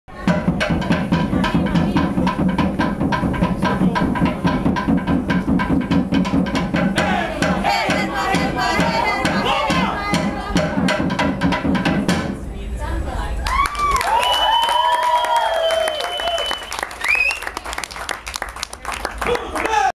Tahitian Dancing by Tropical Production - Flamingo Cantina
Tropical Productions' opening for TAVANA at the Flamingo Cantina, Austin, TX